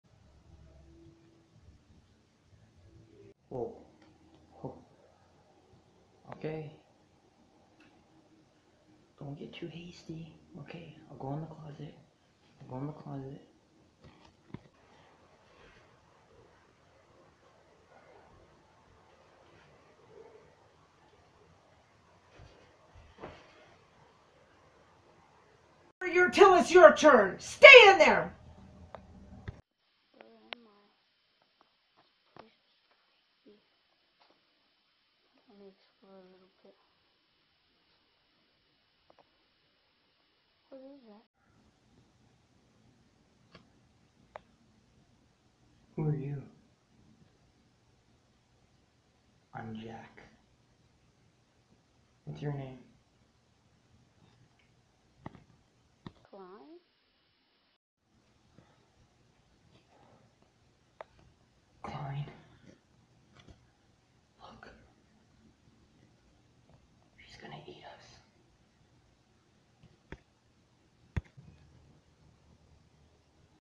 Fear the Dead, Season 7. Ep:8
Linda locks up Jack, and Klein and Jack meet. In the next episode which is the finale two or more people WILL die (Maybe Xavier) Starring Tyler Riedel and Kim Riedel and Logan Gonzales and Produced by Frazar Movie Productions.